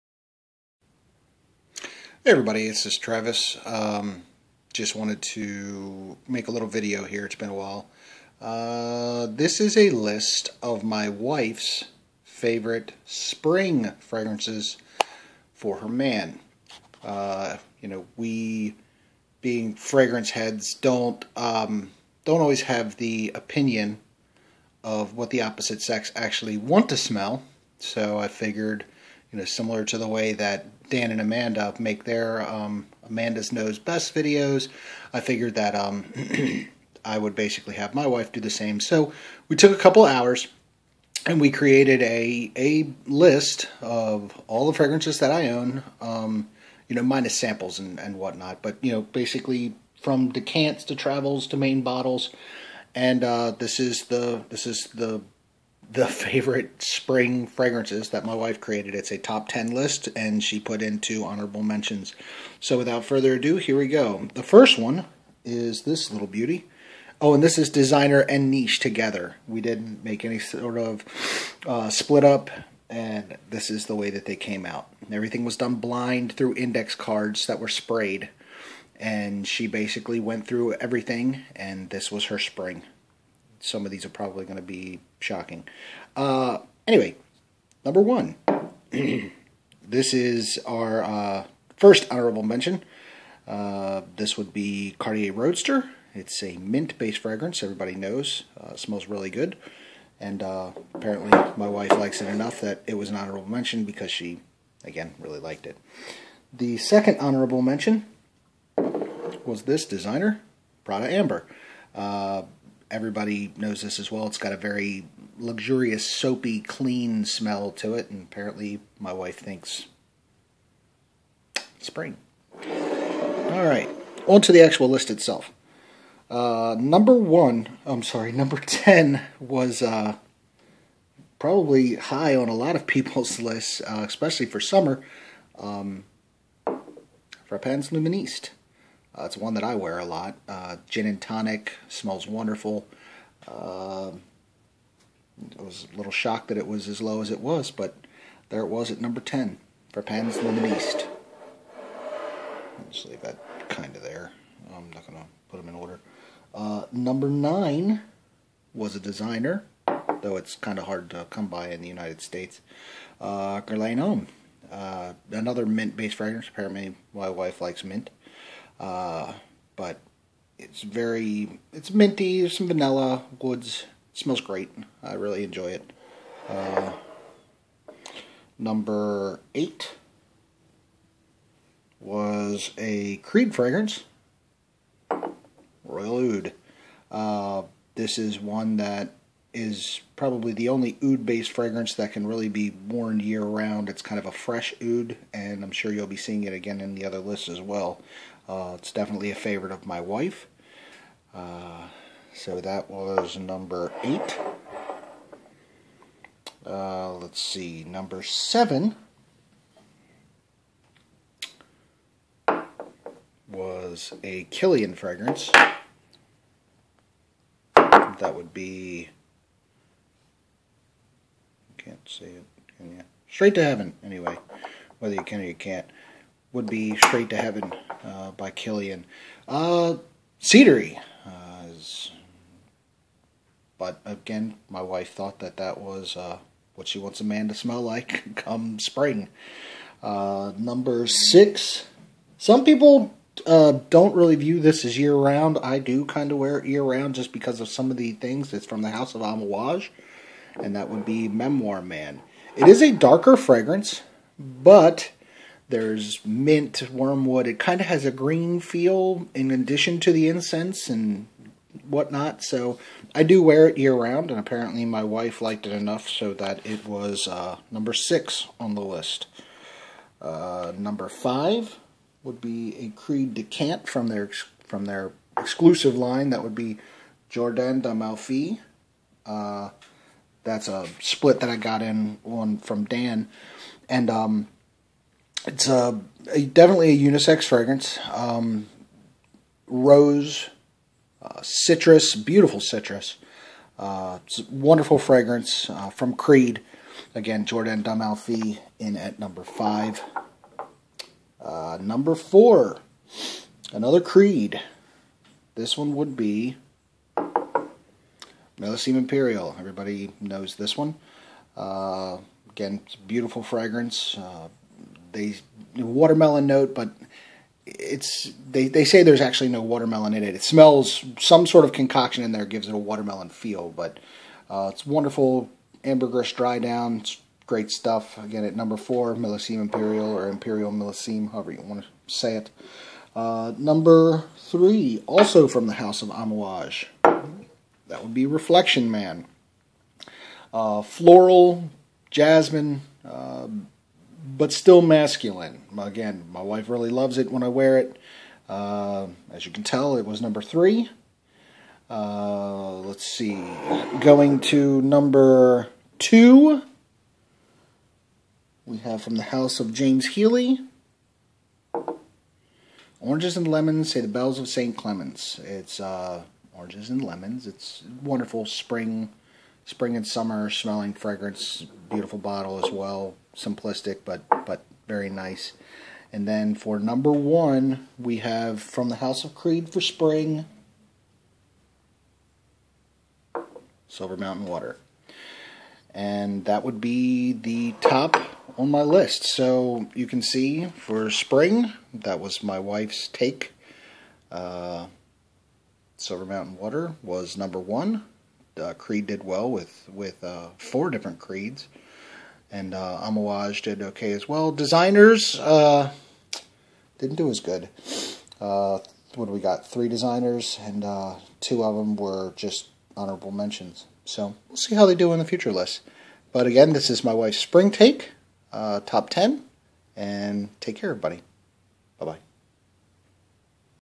Top 10 Spring Fragrances For 2013 - Selected By My Wife...
So you think you smell good huh??? Isn't it better to know what women enjoy smelling on their men??? I had my wife take a few hours and blindly sniff all of my designer and niche fragrances in order to chose what she would want a man to smell like in the Spring... Some of the choices may surprise you... Enjoy the 2013 edition of my wife's top 10 Spring fragrances... (Remember... This is a woman's point of view... ^_^)